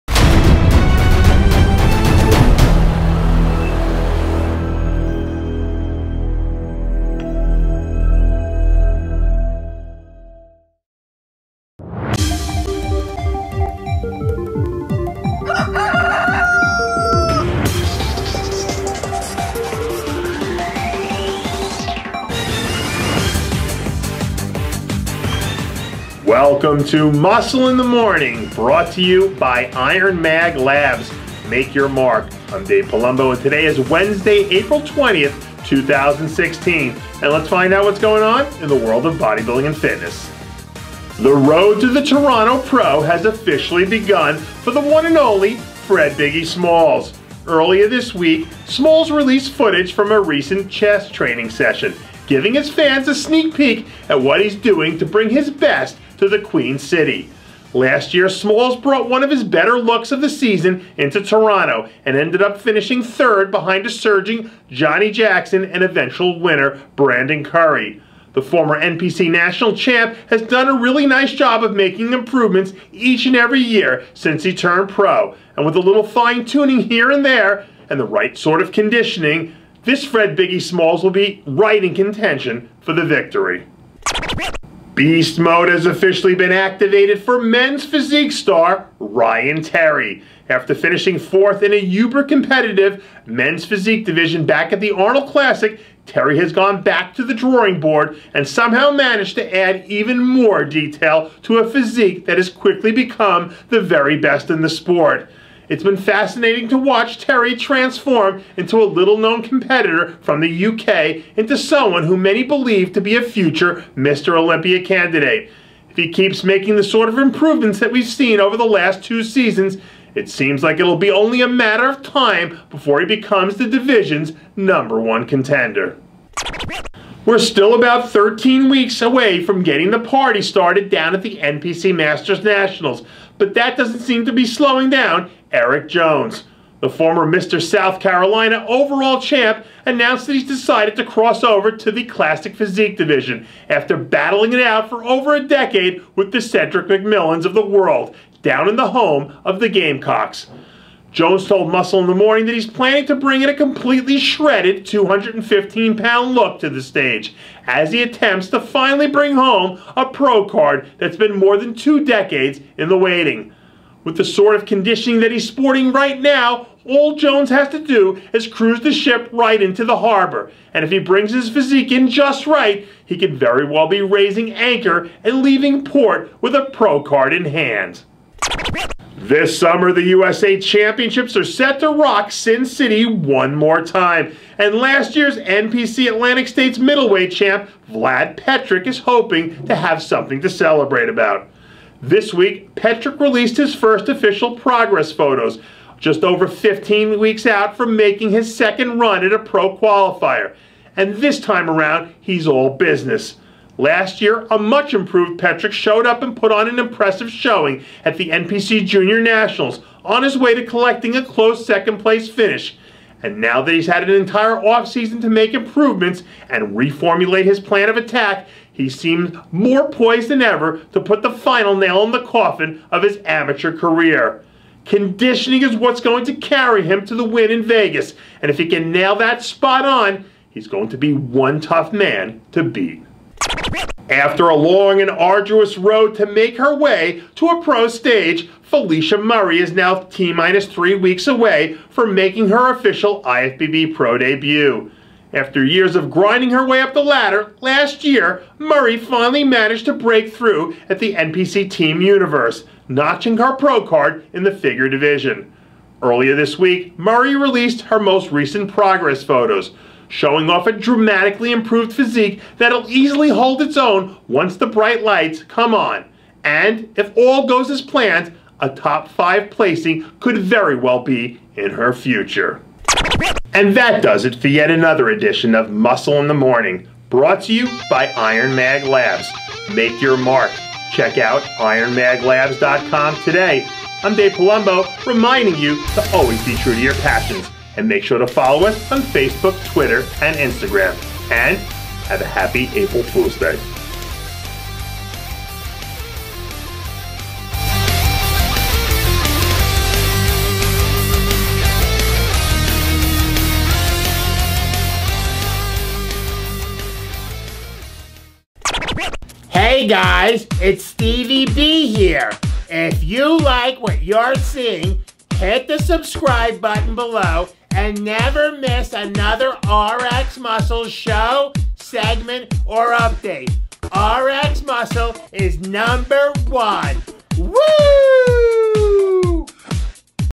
VLAD READY FOR USA ! Muscle In The Morning April 20, 2016
Biggie Smalls Trains Chest, Ryan Terry Shredded, Eric Jones Dialing It In, Vlad Petric 15 Weeks Out, Felecia Murray 3 Weeks Out